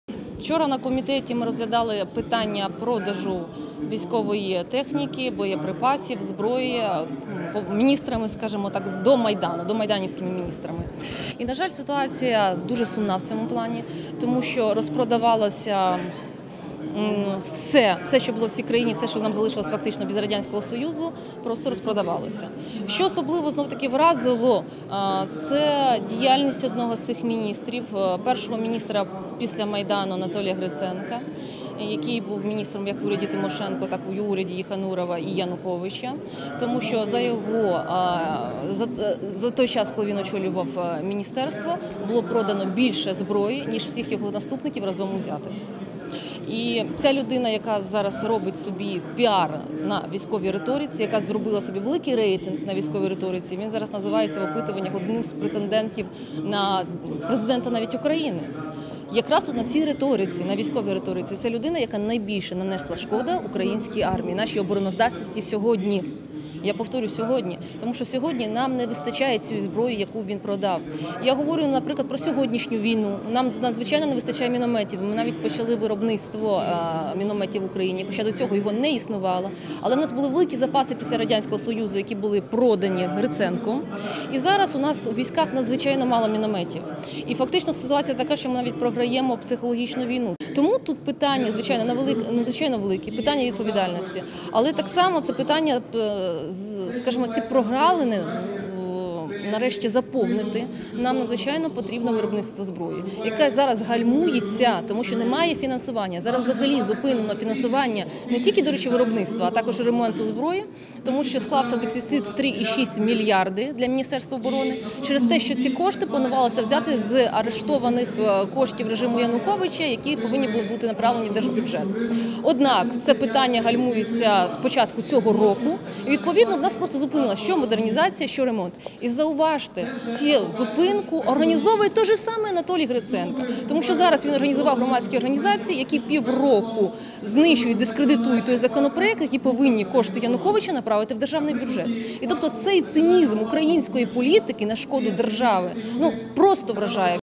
Колишні міністри оборони підірвали обороноздатність і національну безпеку України, - Тетяна Чорновол
Діяльність колишніх міністрів оборони, які обіймали цю посаду до початку військової агресії Росії, суттєво підірвала обороноздатність і національну безпеку України.

Про це сьогодні заявила народний депутат фракції “Народний фронт” Тетяна Чорновол у коментарях журналістам.

Вона повідомила, що учора Комітет з питань національної безпеки і оборони розглядав питання масового продажу військової зброї та техніки міністрами, які працювали до 2014 року, і дійшов висновку, що “розпродавалось все, що лишилося цій країні від Радянського союзу”.

“Що особливо вразило, то це діяльність одного з цих міністрів після революції 2004 року - Анатолія Гриценка, який був міністром у першому Уряді Юлії Тимошенко та урядах Юрія Єханурова і Януковича. Оскільки за той час, коли він очолював міністерство, зброї було продано більше, аніж усіма його наступниками разом узятими”, - сказала Чорновол.

Народний депутат зазначила, що Анатолій Гриценко “зараз робить собі активний піар і високий рейтинг на військовій риториці, навіть називає себе одним із претендентів на посаду президента України”.

“Це людина, яка найбільше нанесла шкоди українській армії. Оскільки сьогодні нам не вистачає зброї, яку він продав, наприклад, мінометів. Були великі запаси, які лишилися від Радянського союзу, а він їх продав”, - розповіла народний депутат.

“Зараз головне питання, щоб ці прогалини були заповнені. Нам надзвичайно необхідно поповнювати запаси зброї. Але, на жаль, це гальмується, оскільки ми не маємо фінансування на нові озброєння і ремонт техніки. У бюджеті Міноборони зараз склався дефіцит в 3,6 мільярда через те, що ці кошти мали бути взяті з арештованих коштів режиму Януковича”, - сказала Чорновол.

За її словами, до того, що гальмується ухвалення відповідного закону, теж доклався колишній міністр Гриценко, який через дружні до нього громадські організації критикує і гальмує ухвалення закону про спецконфіскацію.

“Цей цинізм української політики на шкоду держави просто вражає”, - резюмувала Чорновол.

Приєднуйся!